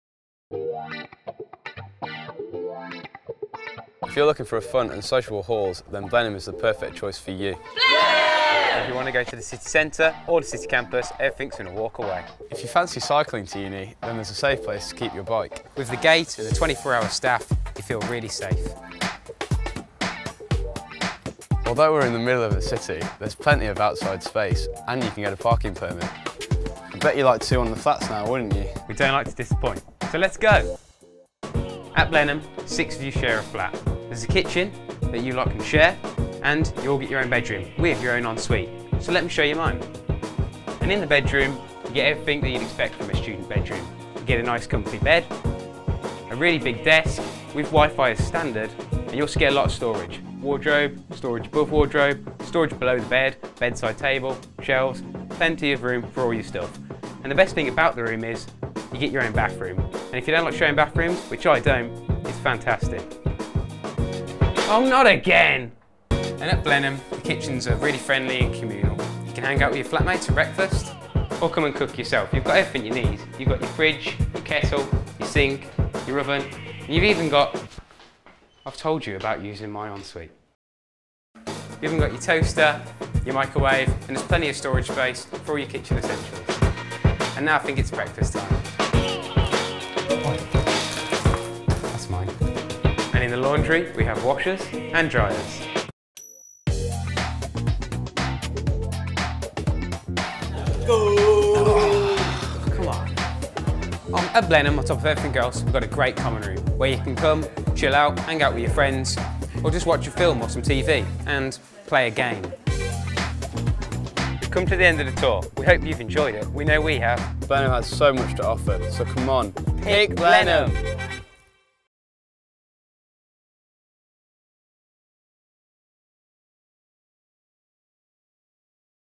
Blenheim - NTU City Campus Accommodation
Our students give you a tour of Blenheim, an attractive & unique crescent-shaped residence in landscaped grounds near the City site.

Visit our Accommodation pages on our website for even more information and details of how to book